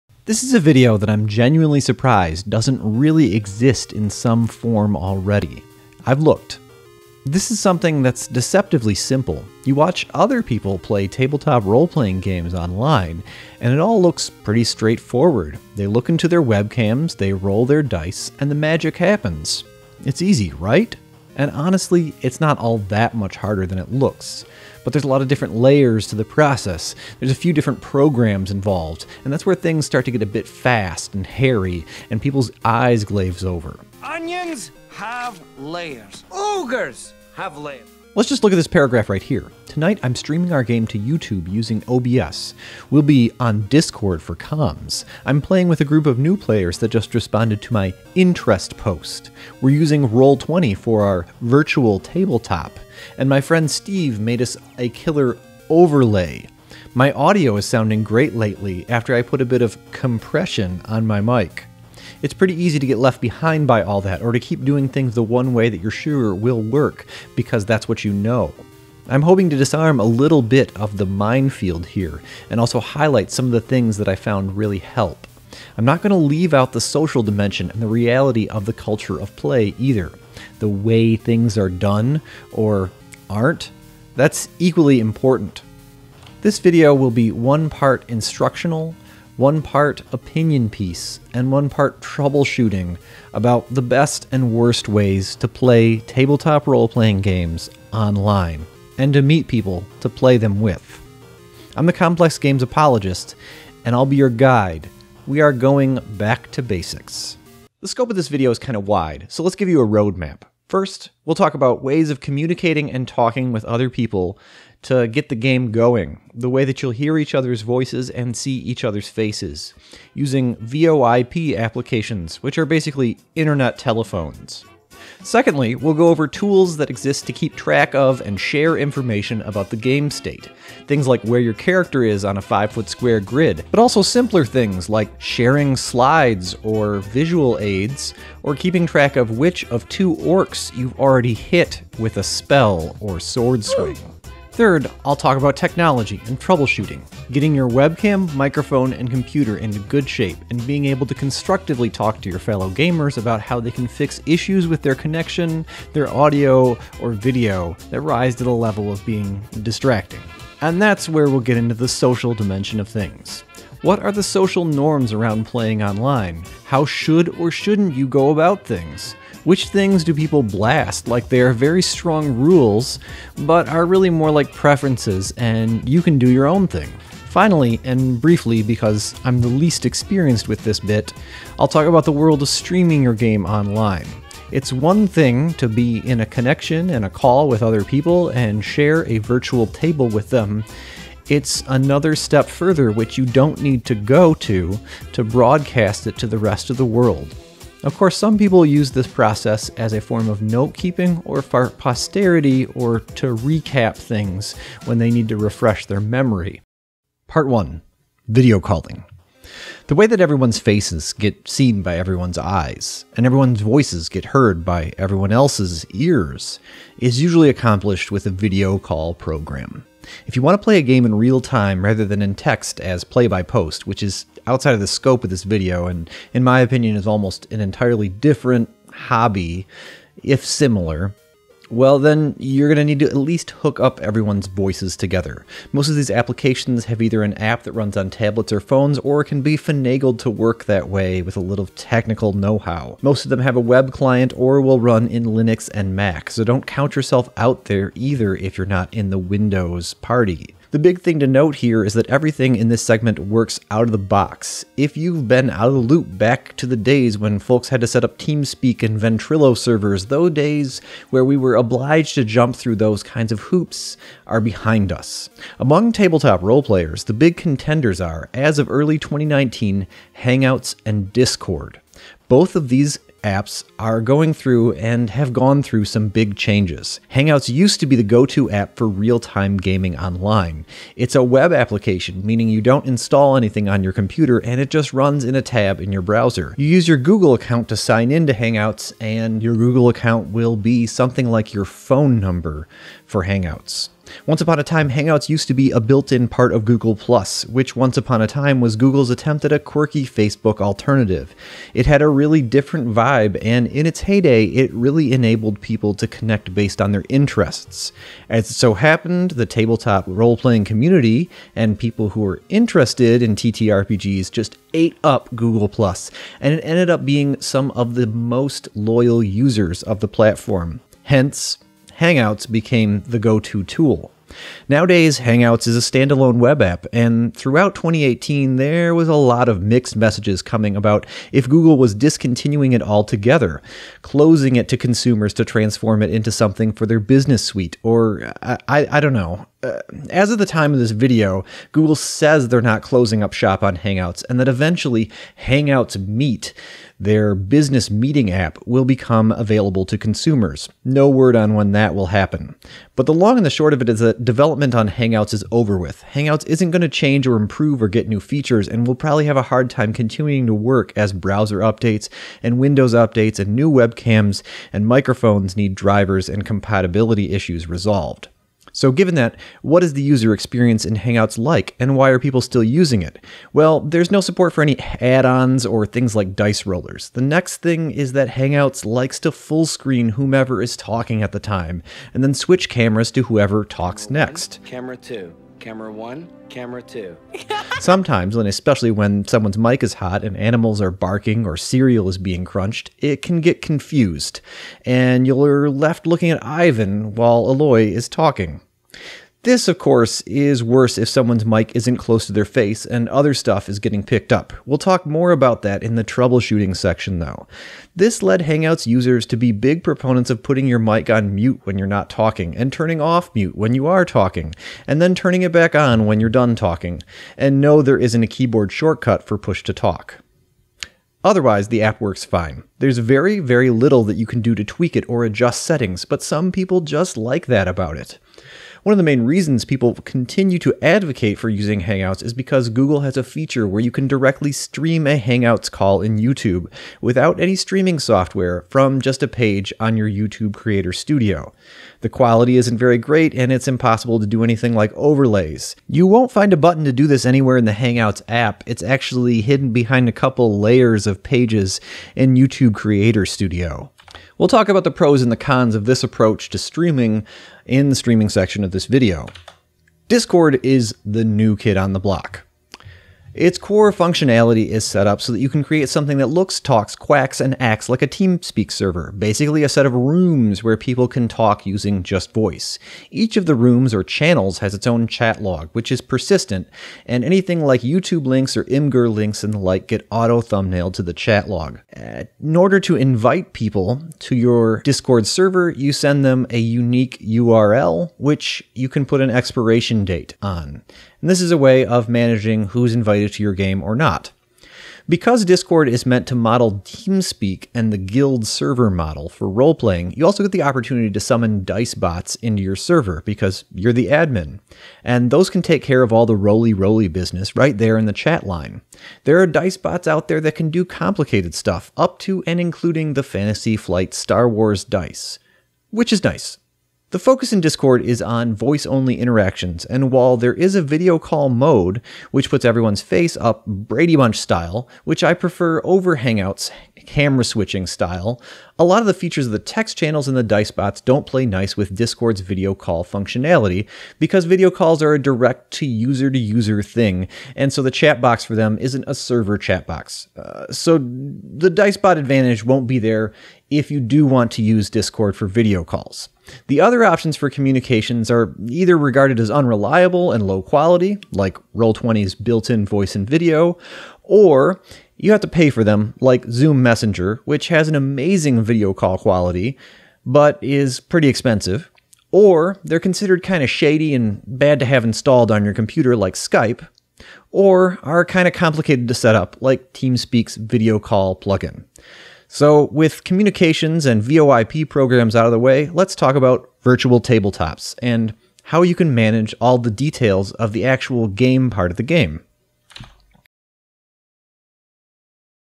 How to Play RPGs and D&D online: Step 1
For this video, I assume that you, the viewer, have a passing familiarity with tabletop RPGs, but are looking to dscover the world of playing and running games online.  In this installment, I'll talk over the basic gatekeeper application to begin the process: you need to to be able to talk to each other to have a live session.  Which programs are options, and why should you pick one over another?

This is the gateway to playing Tabletop Games when you can't get a good face to face option, and it's a way to play different systems and different kinds of campaigns than you might otherwise be able to.  It's a chance to play if you only GM with your regular group,  and Vice Versa.

Look for Regular Episodes of The Complete Guide here on the channel in 2019.  Happy New year!

a great many of these icons are from Flatico.  For a full list of the Creative Commons Credits, please see this document